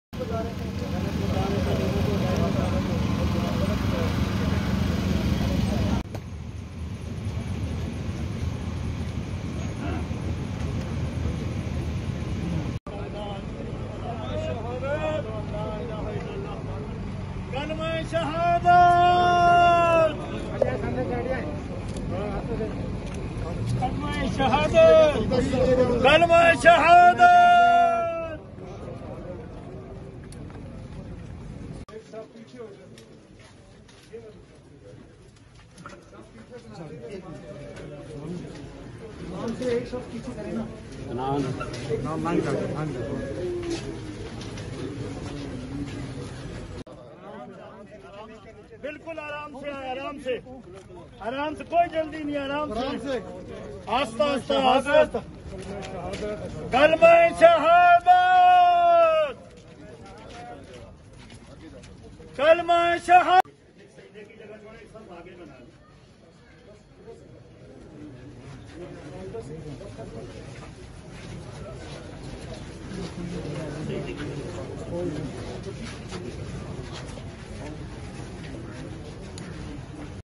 Funeral prayers of Uncle Sargam Farooq Qaiser were offered at H-11 graveyard Islamabad.
معروف فنکار و ہدایتکار فاروق قیصر المعروف انکل سرگم کی نماز جنازہ ادا کردی گئی۔آئی جی موٹر وے پولیس کلیم امام۔پیپلزپارٹی کے رہنما فرحت اللہ بابر۔جماعت اسلامی کے نائب امیر میاں اسلم فرحت اللہ بابر سمیت فنکاروں اور دیگر لوگوں کی بڑی تعداد میں جنازے شرکت کی۔